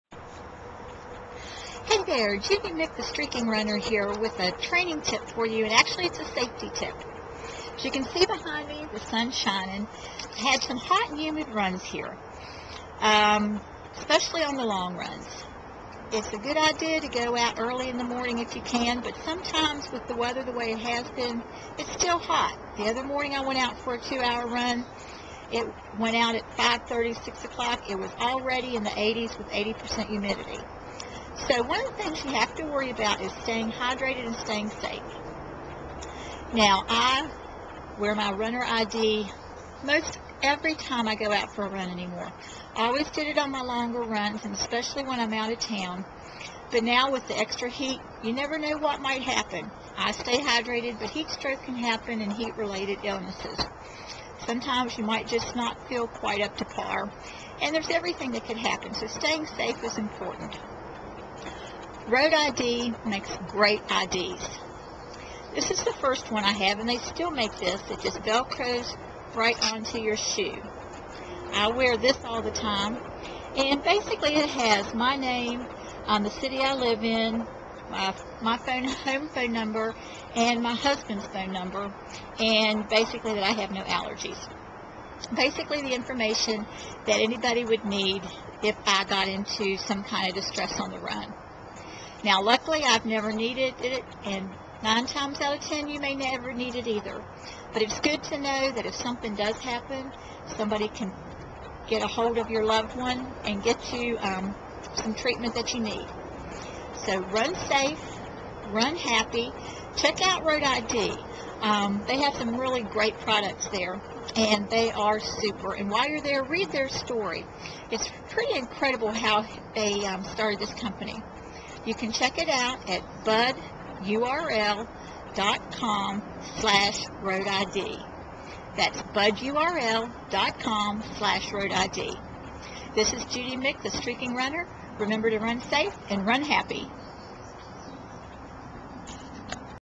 Running Tips - Carrying ID While Running Is Important
It's important to have ID on you when you run.  It's especially important when you run on extremely hot and humid days.